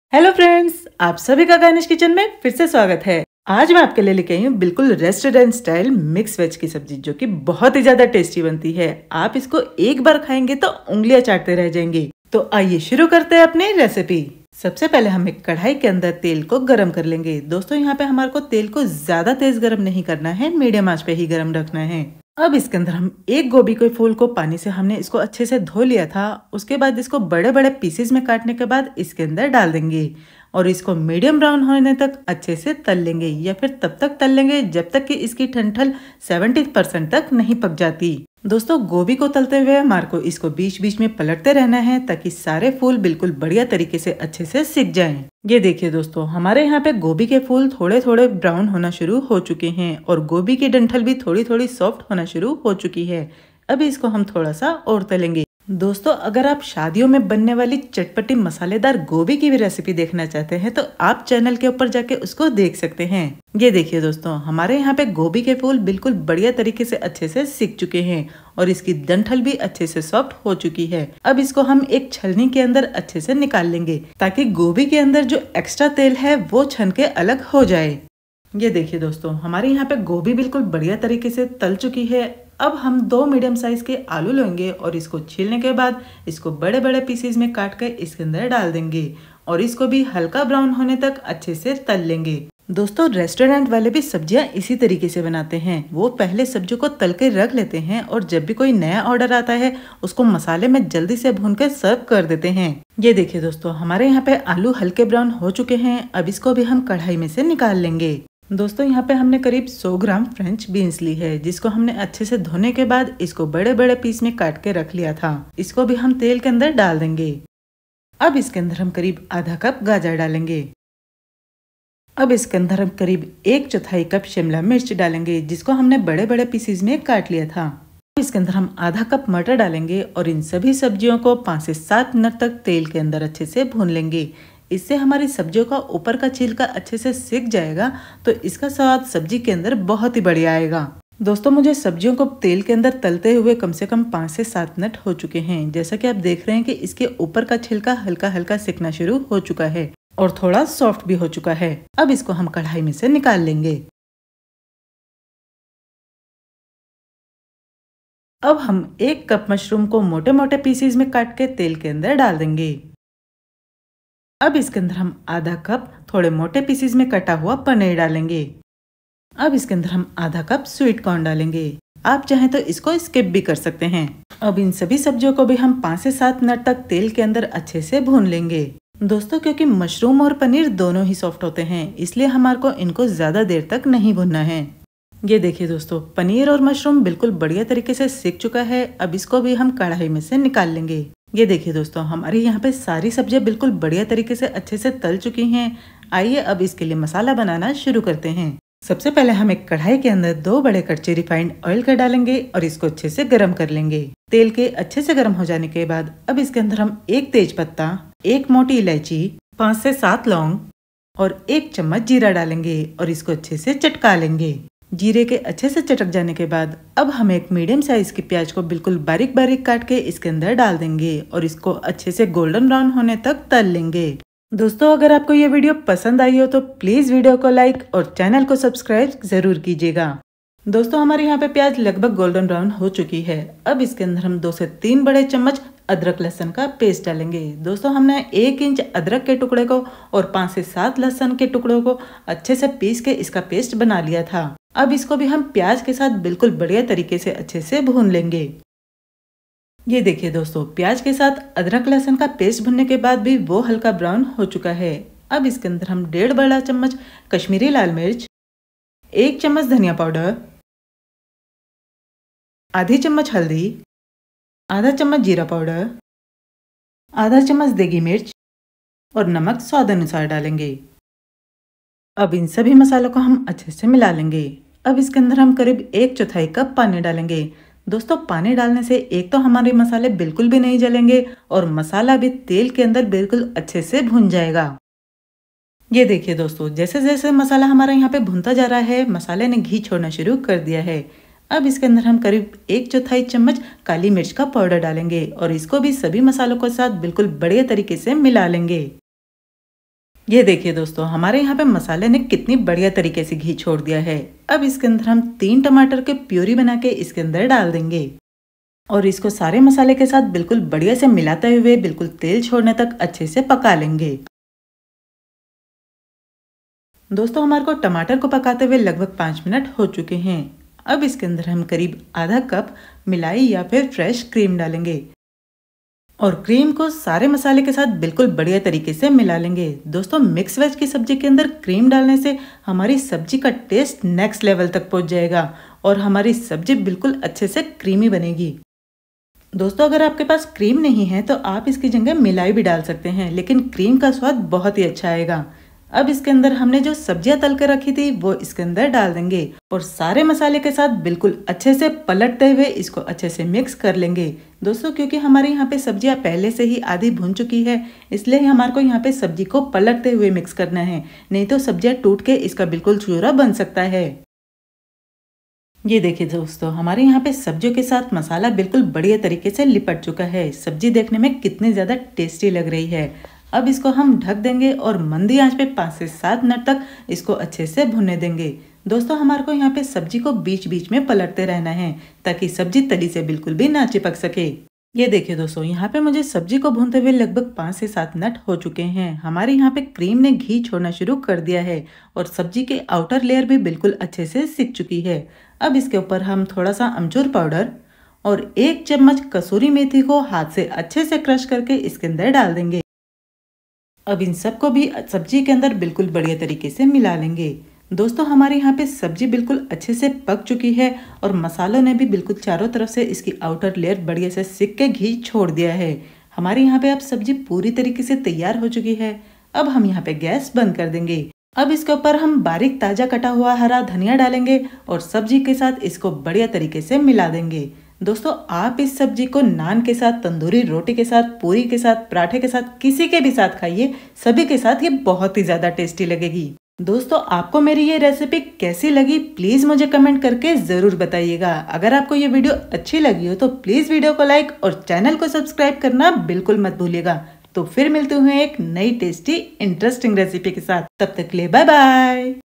Mix Veg Recipe | मिक्स वेज रेसिपी | Mix Vegetable Recipe | Restaurant Style Mix Veg Recipe
Ingredients :
1 फूल गोभी
1 गाजर
1/2 कप मटर
मशरूम
शिमला मिर्च
स्वीट कॉर्न
प्याज़
3 टमाटर
अदरक लहसुन पेस्ट
हरा धनिया
पनीर
2 आलू
बीन्स
रिफाइंड तेल
तेज पत्ता
लौंग
काली इलाइची
1 Tsp जीरा
1+1/2 Tbsp कश्मीरी मिर्च
1/2 Tsp देगी मिर्च
1/2 Tsp हल्दी
नमक
1 Tsp कसूरी मेथी
1 Tsp धनिया पाउडर
1/4 Tsp काली मिर्च पाउडर
1/3 Tsp खटाई